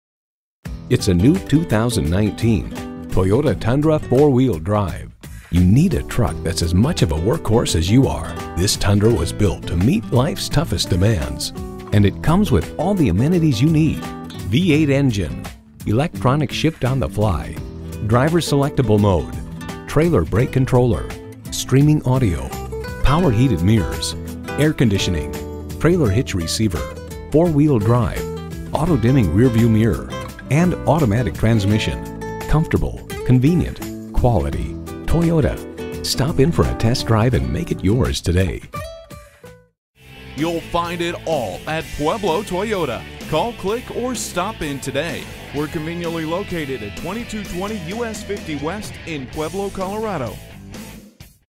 New 2019 Toyota Tundra Pueblo CO Colorado Springs, CO #198246 - SOLD
Year: 2019
Make: Toyota
Model: Tundra
Trim: SR5 5.7L V8
Engine: 5.7L V8
Transmission: Automatic 6-Speed
Color: Magnetic Gray
Stock #: 198246
VIN: 5TFDY5F10KX831225

Pueblo Toyota
2220 US-50 West
Pueblo, CO 81008

Address:
2220 US-50 West